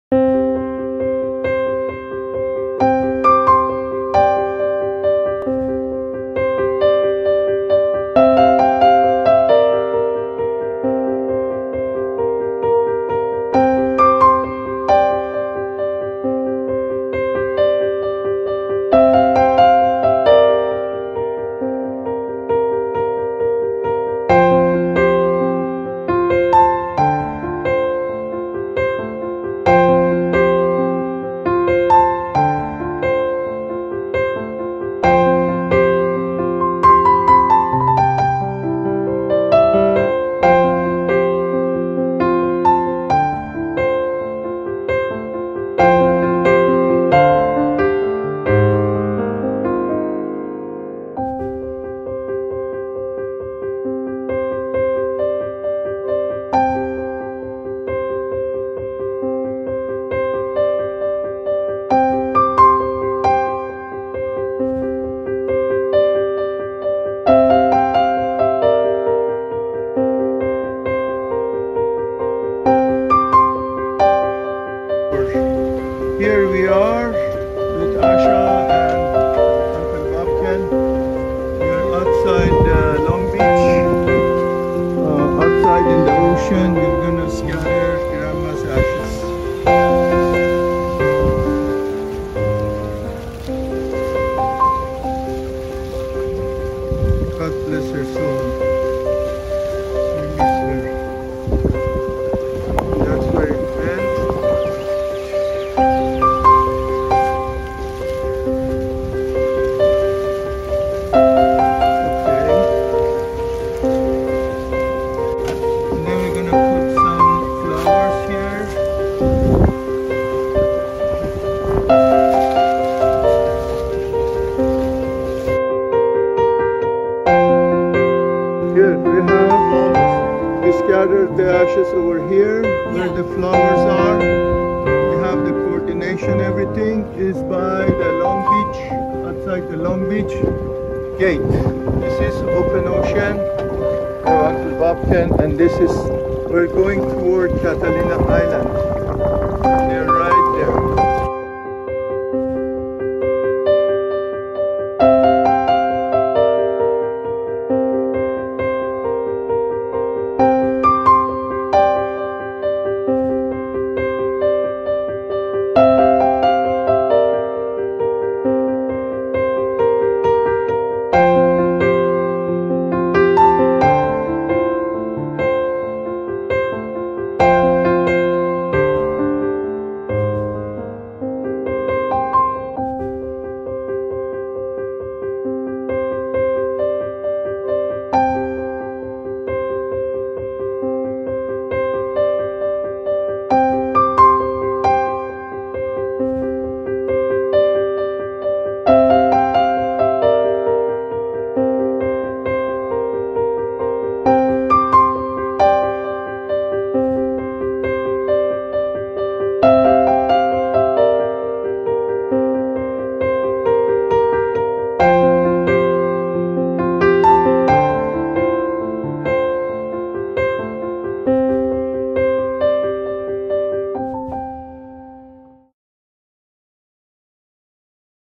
Grandma Shobha's Scattering Ashes
In Pacific Ocean, off the cost of Long Beach, CA, on Mon. Feb/28/2022, with Asha, Bagrad and Uncle Babken, Latitude: 33°43'18.2" N
Longitude: 118°10'49.5"W
Copy & Paste the following coordinate into your google map search box for the exact location: 33°43'18.2"N 118°10'49.5"W